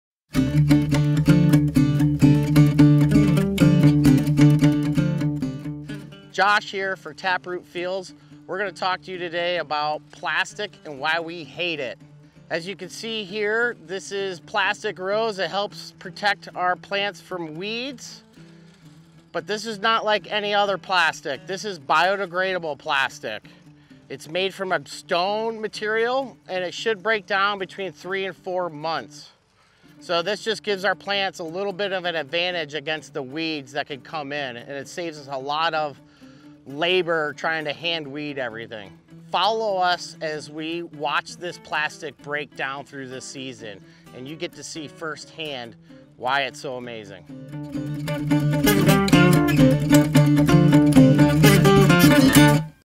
Tap Root Fields: Biodegradable Plastic Mulch Farming Film
To help manage weeds in our fields we use a biodegradable plastic mulch film. Great for the environment and our crop!